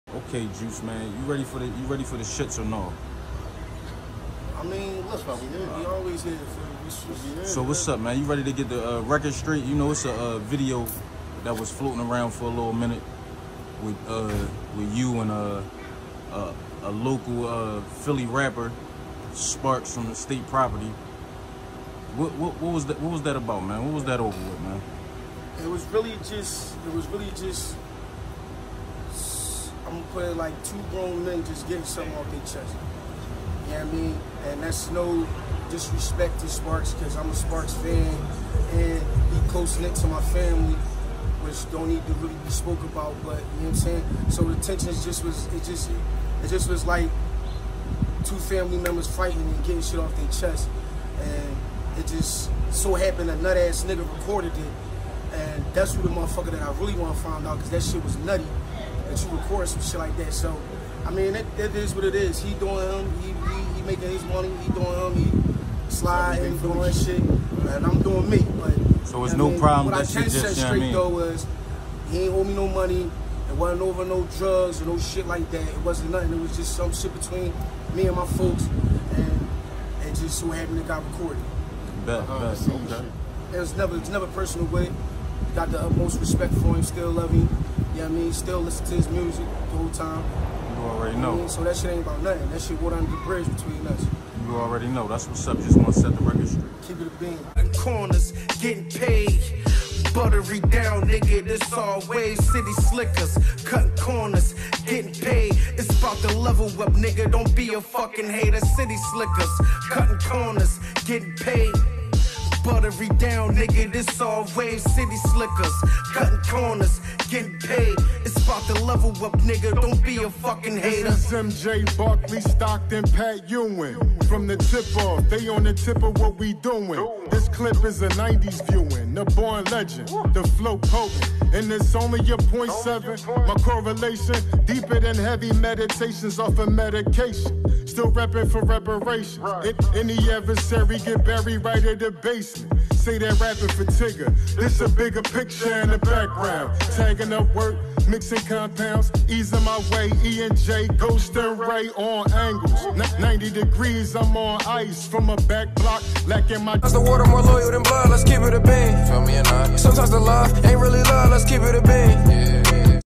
Juice sets the record straight on viral Omillio Sparks Fight on 52nd st. Must watch(GrownManShit)
Sat down with Juice and Ski_wp for a full interview click it to watch in full. But if you want to get right to it this where he talks about the fight that got outta hand and wasn’t meant to be recorded but got out there.